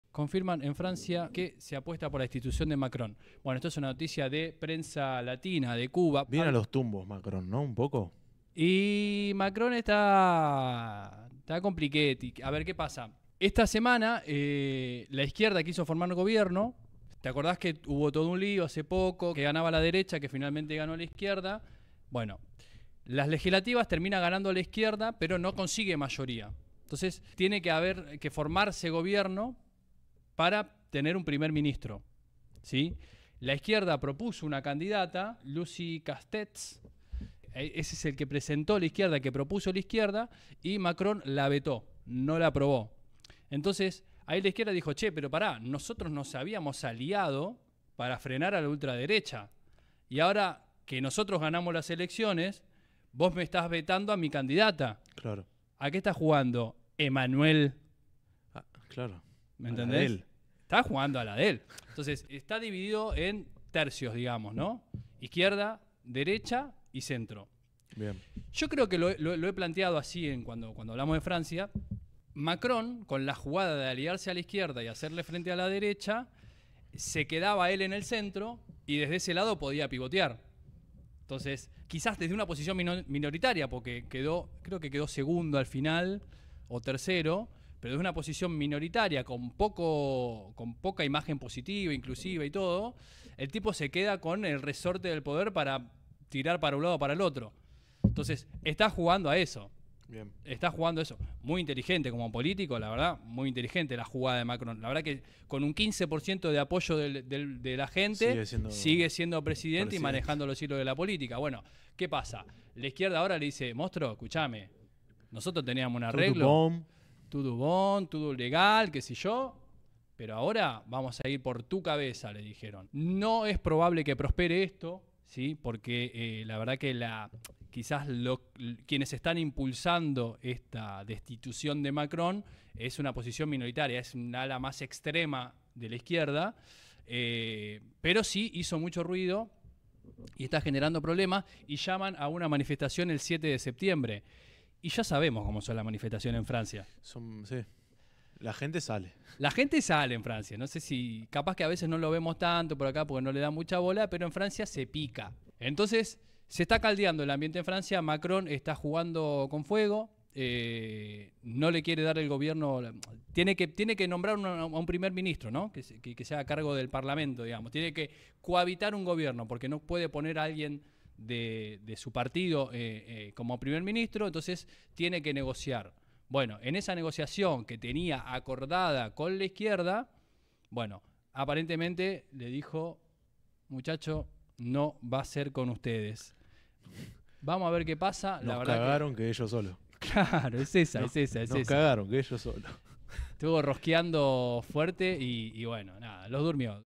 FRANCIA: La izquierda va por la DESTITUCIÓN del PRESIDENTE
Emmanuel Macron se encuentra en el ojo de la tormenta, luego de la victoria de la izquierda en las elecciones legislativas el presidente se rehusa a darle lugar en el puesto de primer ministra a Lucie Castets, quien fue la propuesta por la coalición victoriosa.

A partir de la negativa del presidente La Francia Insumisa propone la destitución de Macron, para ello llama a una manifestación el día 7 de septiembre.

¿A qué juega Macron? desde su lugar "centrista" aprovecha para pivotear entre izquierda y derecha, en definitiva, conserva el poder incluso siendo minoría